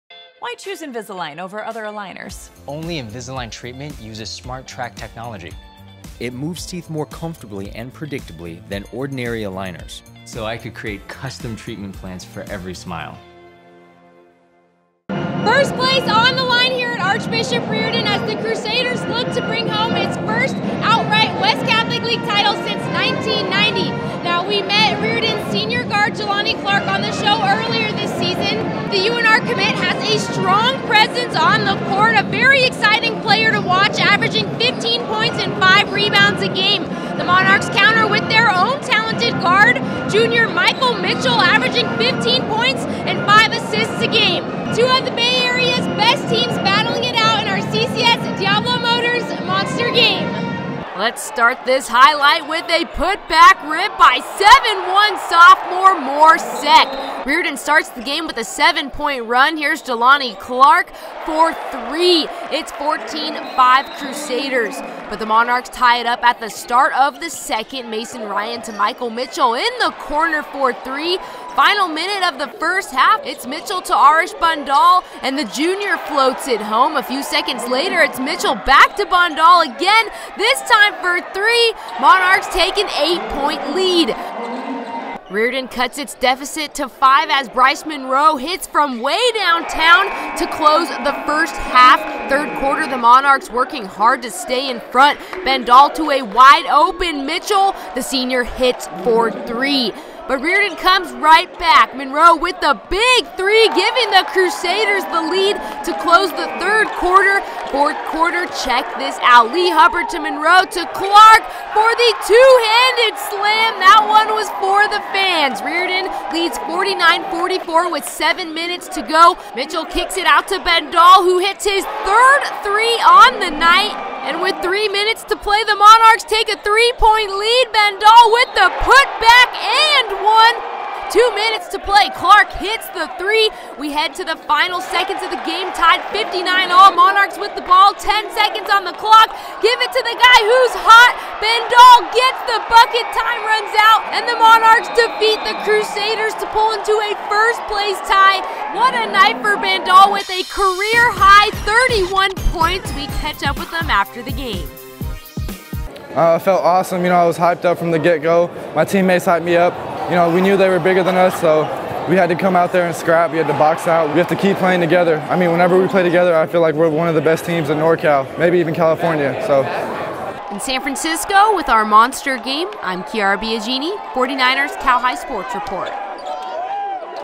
Mitty at Riordan Boys Basketball | Arrish Bhandal vs Bryce Monroe 02.11.20
LET'S START THIS HIGHLIGHT WITH A PUT BACK RIIIIP BY 7'1 SOPHMORE MOR SECK! RIORDAN STARTS THE GAME WITH A 7 POINT RUN...

HERE'S JE'LANI CLARK PULLING UP BEYOND THE ARCH... MONEY! ITS 14-5 CRUSADERS

BUT THE MONARCHS TIE IT UP AT THE START OF THE SECOND... MASON RYAN TO MICHAEL MITCHELL IN THE CORNER FOR THREE!

FINAL MINUTE OF THE FIRST HALF... ITS MITCHELL TO ARRISH BHANDAL AND THE JUNIOR FLOATS IT HOME...

A FEW SECONDS LATER ITS MITCHELL BACK TO BHANDAL AGAIN THIS TIME FOR THREE! MONARCHS TAKE AN 8 POINT LEAD

BUT RIORDAN CUTS ITS DEFECIET TO 5 AS BRYCE MONROE HITS FROM WAAAAAY DOWNTOWN TO CLOSER THE FIRST HALF..

THIRD QUARTER THE MONARCHS WORKING HARD TO STAY IN FRONT... BHANDAL TO A WIDE OPEN MITCHELL... THE SENIOR HITS FOR THREE...

BUT RIORDAN COMES RIGHT BACK... MONROE WITH THE BIG THREE GIVING THE CRUSADERS THE LEAD TO CLOSE THE THIRD QUARTER...

FOURTH QUARTER... CHECK THIS OUT... LEE HUBBARD TO MONROE TO CLARK FOR THE TWO HANDED SLAM! THAT ONE WAS FOR THE FANS! RIORDAN LEADS 49-44 WITH 7 MINUTES TO PLAY

 A LOT OF TIME LEFT FOR THE MONARCHS... MITCHELL KICKS IT OUT TO BHANDAL HE HITS HIS THIRD THREE ON THE NIGHT!

AND WITH 3 MINUTES TO PLAY THE MONARCHS TAKE A 3 POINT LEAD!! BHANDAL WITH THE PUT BACK AND 1! THIS KID IS HAVING HIMSELF A NIGHT!

2 MINUTES TO PLAY! CLARK HITS THE THREE! WE HEAD TO THE FINAL SECONDS OF THE GAME TIED 59 ALL... MONARCHS WITH THE BALL WITH 10 SECONDS ON THE CLOCK... GIVE IT TO THE GUY WHO'S HOT! BHANDAL GETS THE BUCKET! TIME RUNS OUT AND THE MONARCHS DEFEAT RIORDAN BY 2 POINTS TO PULL INTO A FIRST PLACE TIE... BHANDAL THE DIFFERENCE IN  THIS ONE... WE TALK TO THE MVP AFTER THE GAME...